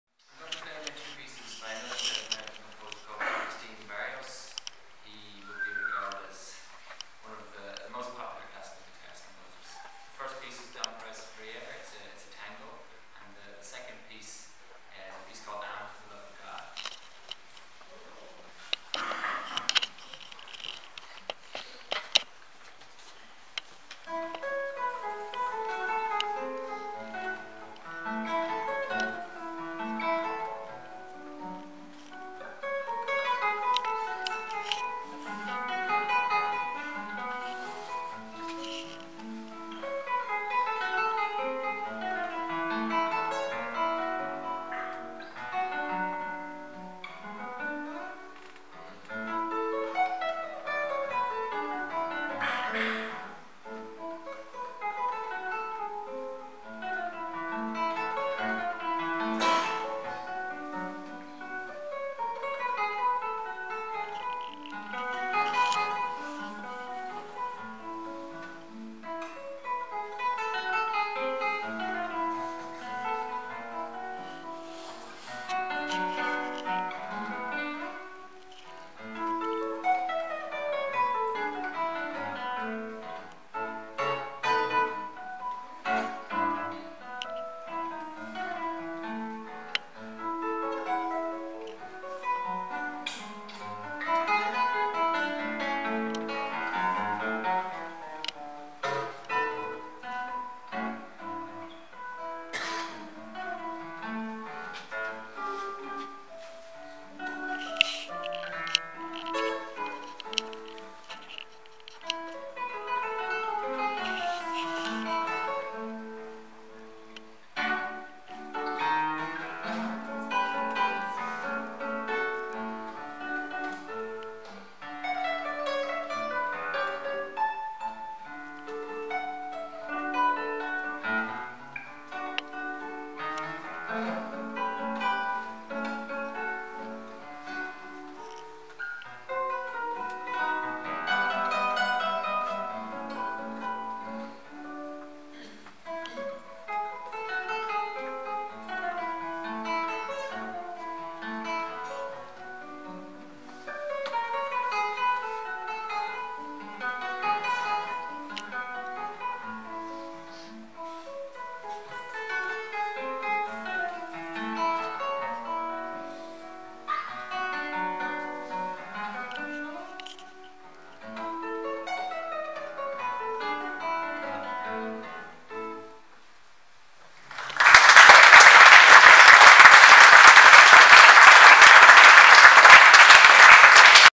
Eoin Flood
Eoin Flood, a professional classical guitarist based in Dublin. He recently graduated with a professional studies diploma from the Manhattan school of Music New York.